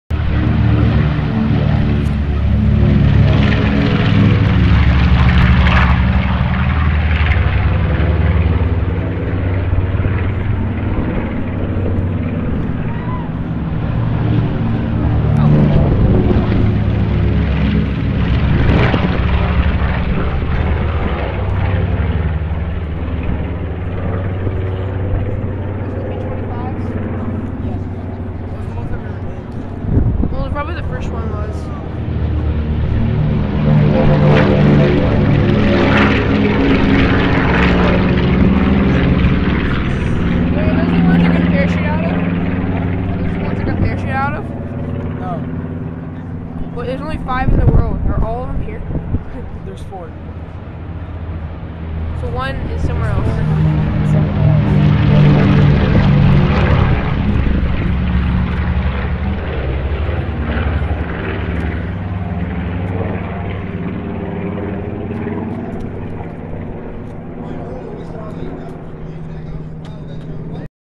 Air Show-Yet, Another B-25 Video
It is just the 4 out of 5 B-25 bomber planes taking off. There are 5 left in the world and there were 4 there so. yeah